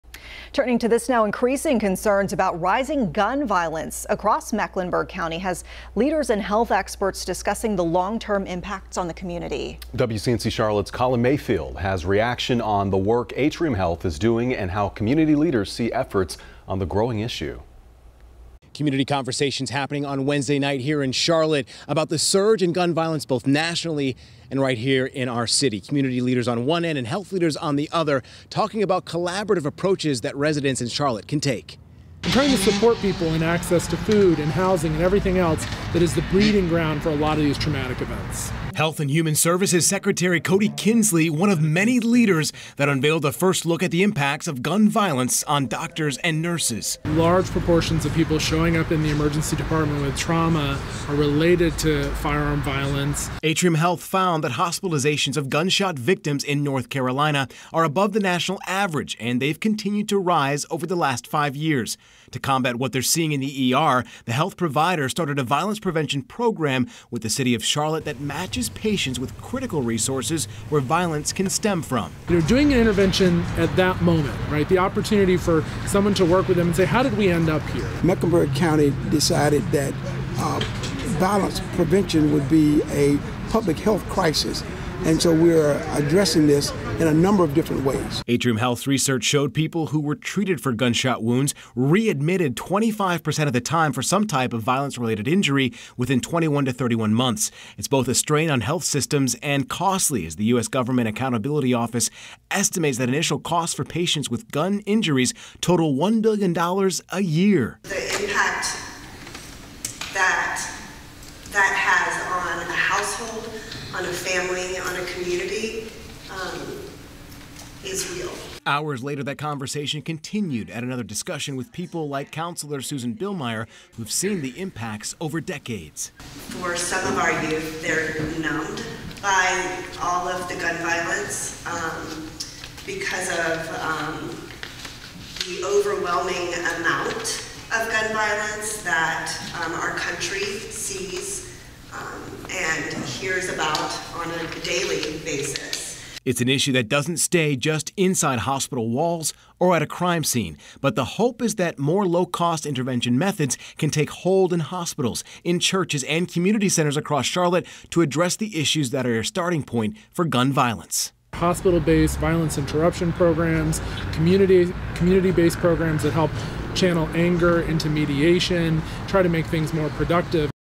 Atrium Health shares the impact gun violence has on health care
Increasing concerns in Charlotte about the rise in gun violence brought Mecklenburg County leaders and health experts together to discuss the long-term impacts on the community.

Atrium Health hosted the discussion Wednesday to explain their approach to how gun violence creates trauma long after doctors and nurses have treated a patient.

Gun violence and its effects were also the topics in a panel of grassroots efforts from community members who are on the ground every day.

Atrium found that hospitalizations of gunshot victims in North Carolina are above the national average and they've continued to go up over the last five years. To combat what they're seeing in the ER, the health provider started a violence prevention program with the city of Charlotte that matches patients with critical resources where violence can stem from.

"We need to support people who are needing access to critical necessities like food and housing," Health and Human Services Secretary Kody Kinsley said. "Everything else like that creates a breeding ground for traumatic events."